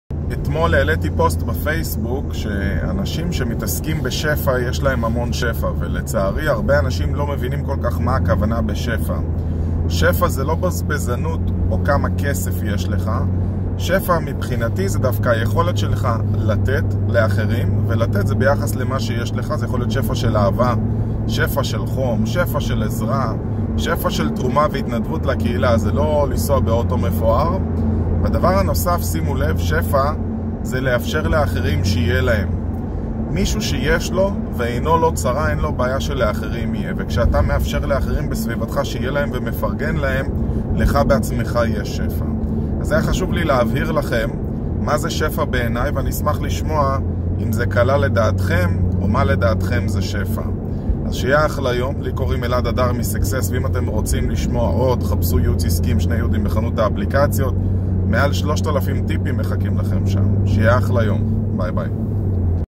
מה ההגדרה שלי לשפע
הבנתי שהרבה אנשים מפרשים שפע לא נכון אז החלטתי להבהיר את הנושא מנקודת המבט שלי